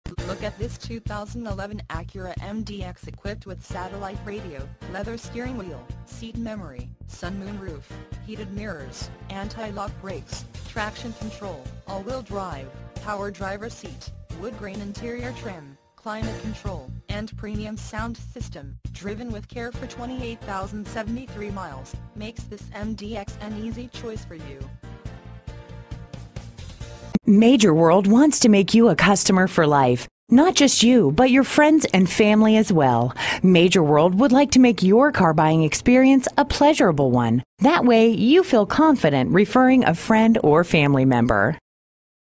used Acura MDX NY New York 2011 located in Long Island City at Major World
This used Acura MDX NY New York 2011 is located in Long Island City and is offered by Major World and can be compared to any used Acura MDX :